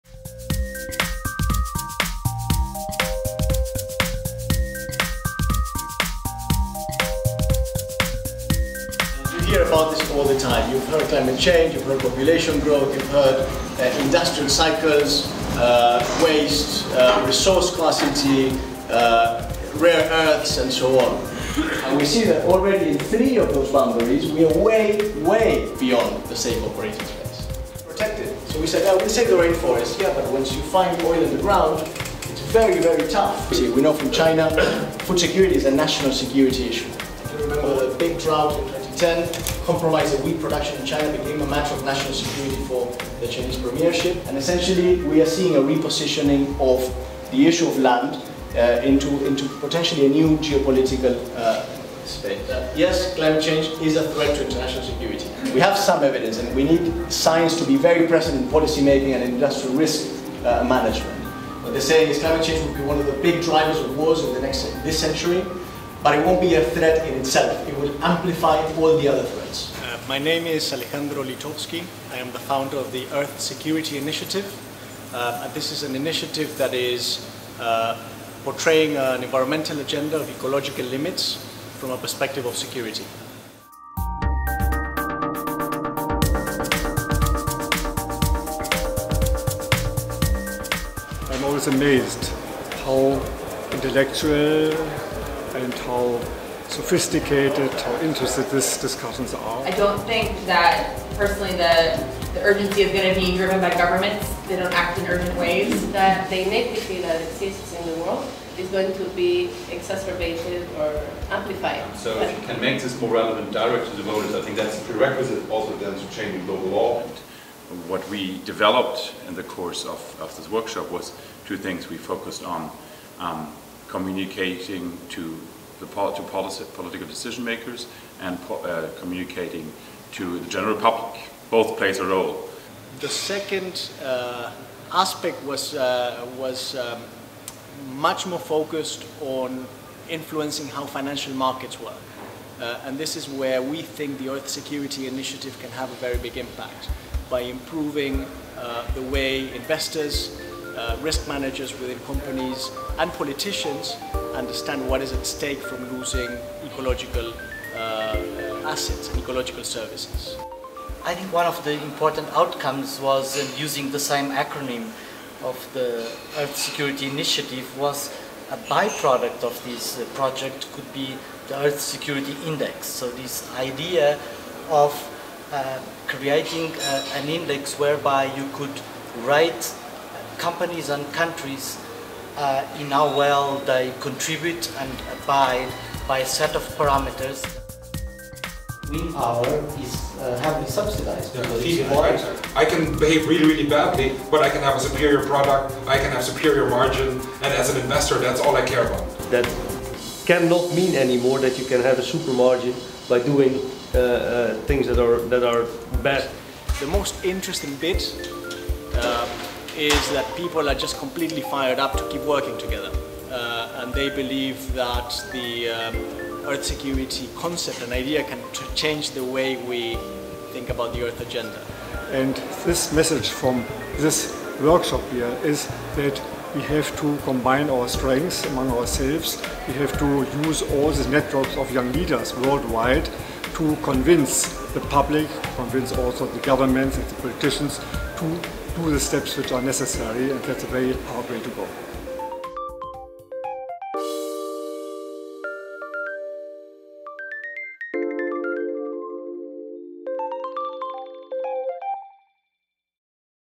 Earth Security Initiative
The ESI defines and communicates the security agenda that is resulting from surpassing the Earth's limits, advocating responses that build a culture of Peace.

Cinematography and interviews: Sven Stegemann
Editing: Stefanie Trambow